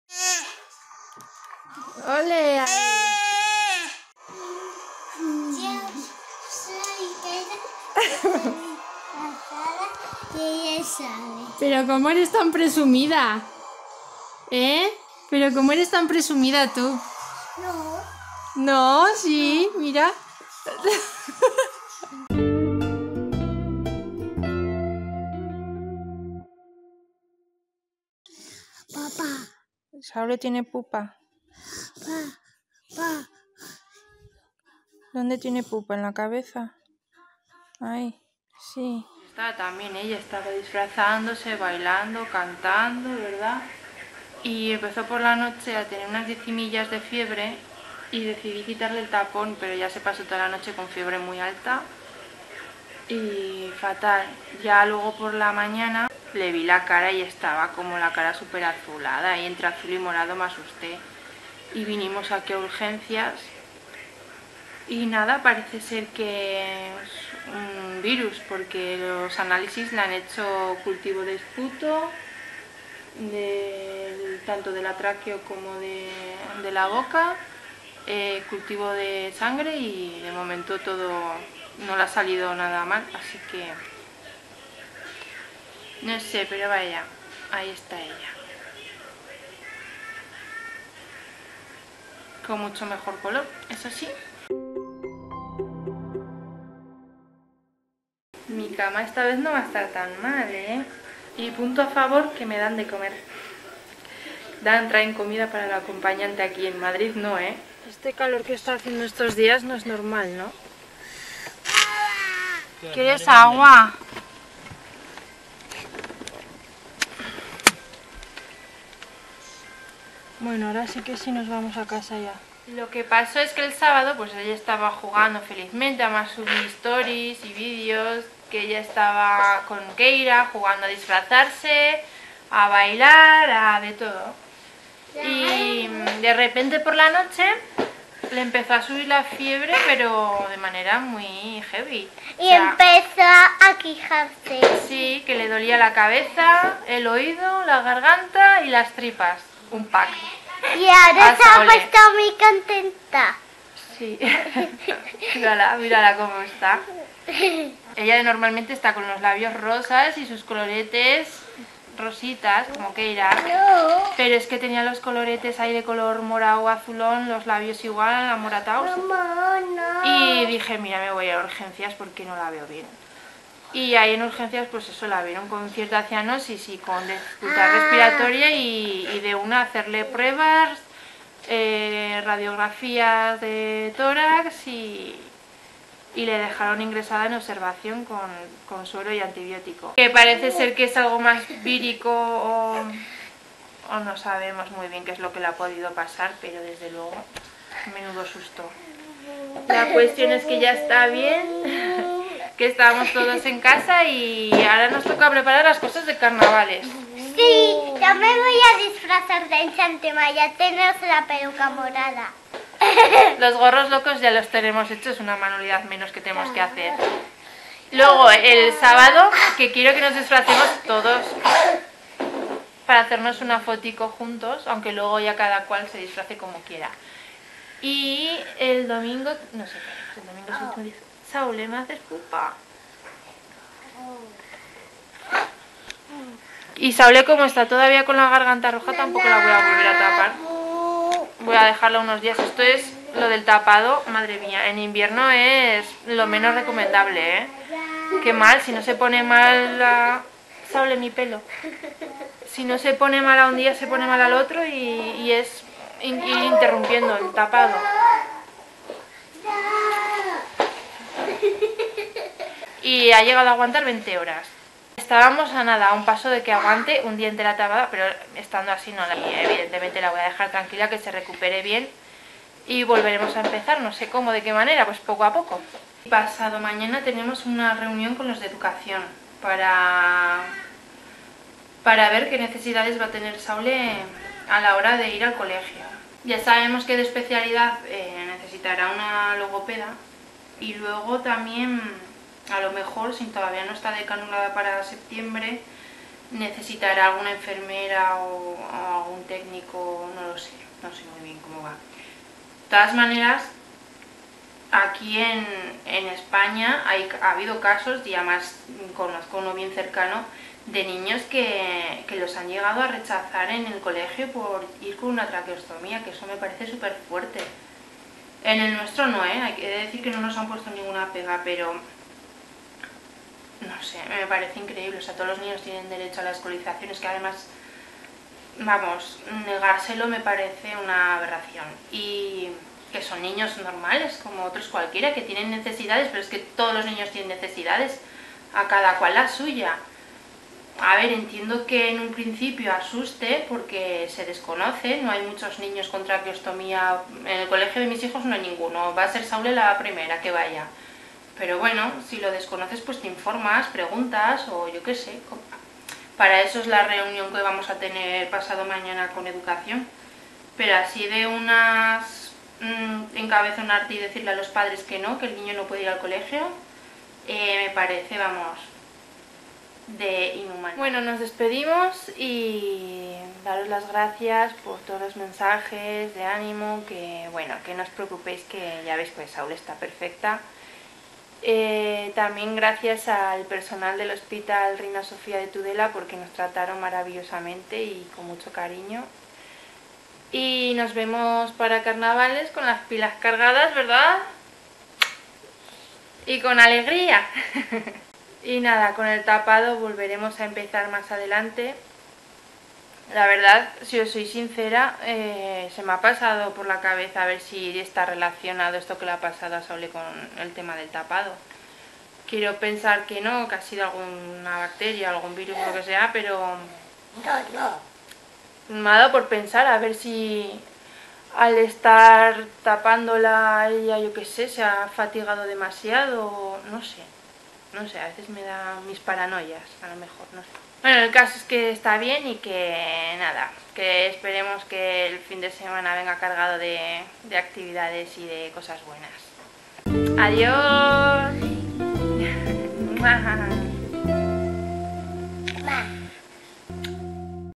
El SUSTO que nos dió SAULE
El fin de semana empezó muy animado pero acabó con un susto y con Saule ingresada en el hospital. Ha sido todo muy extraño, porque pasó de estar muy activa jugando y bailando a estar realmente mal, con episodio de cianosis y dificultad respiratoria incluido y de la noche a la mañana vuelve a estar bien y recuperada. Estoy muy confundida, la verdad y no se que pensar al respecto...